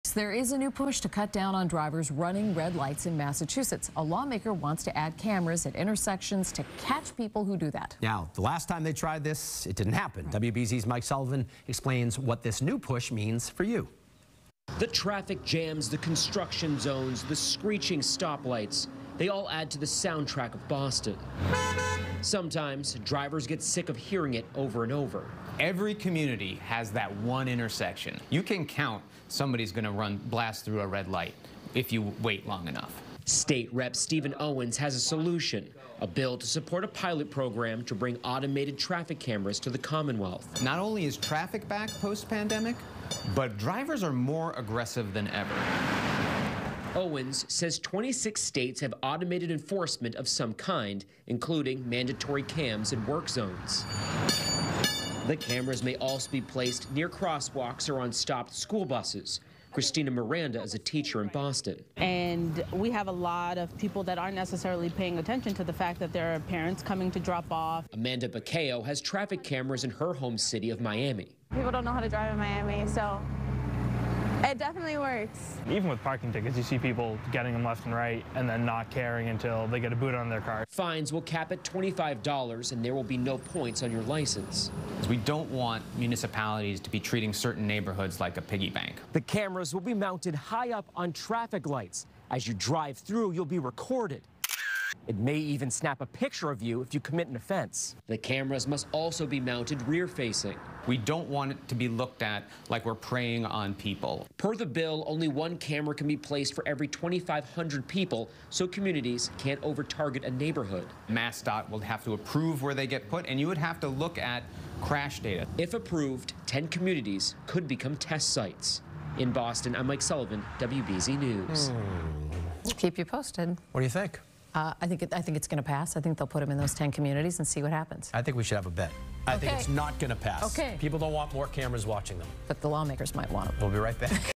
New push to add red light cameras to intersections in Massachusetts
A new bill proposal aims to bring automated traffic enforcement to Massachusetts. WBZ-TV's Mike Sullivan reports.